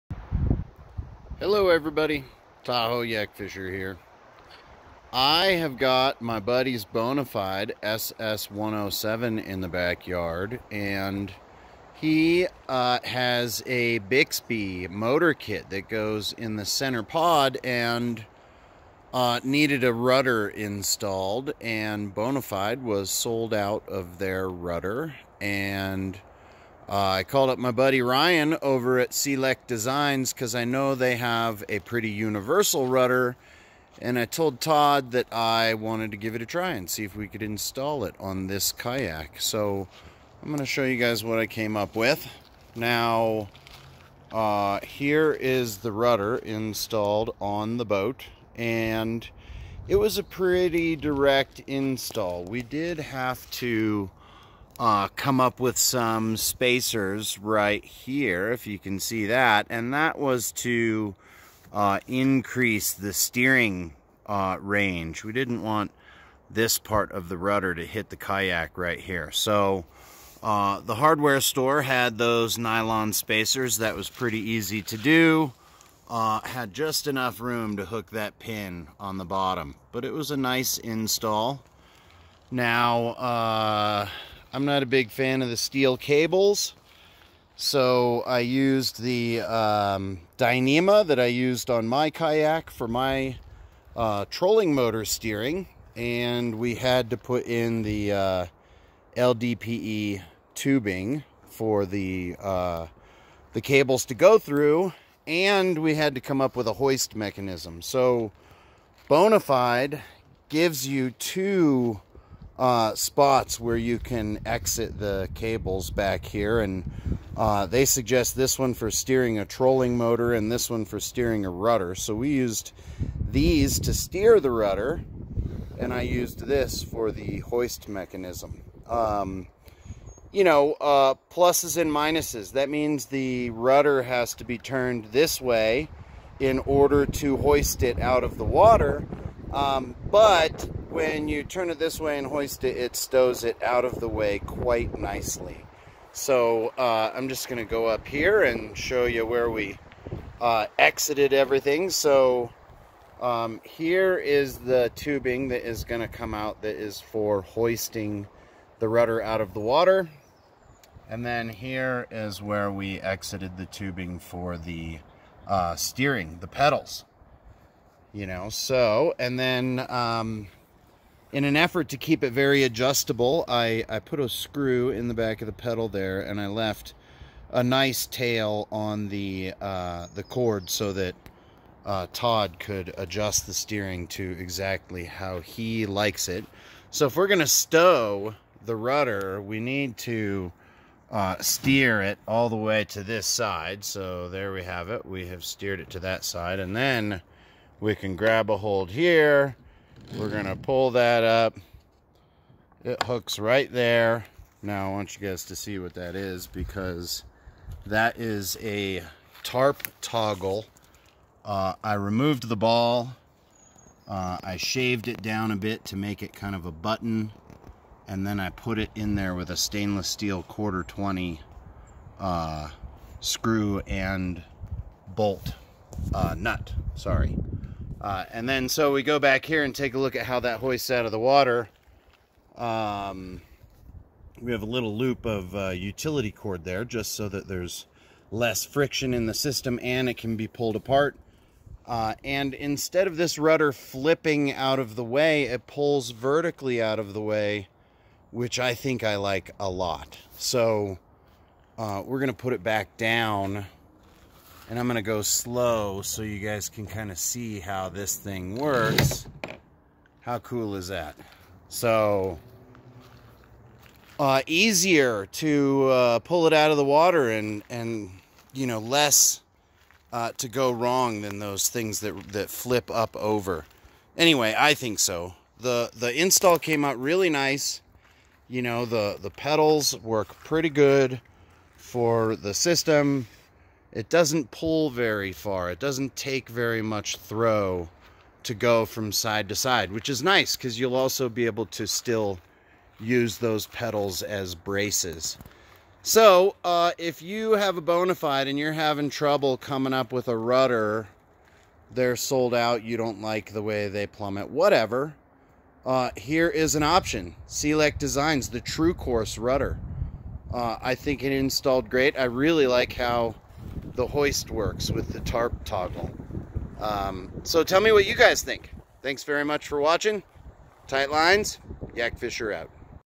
Bonifide SS107 Rudder Install
2021 Bonifide SS107 gets a Sea-lect Designs Trucourse Rudder installed.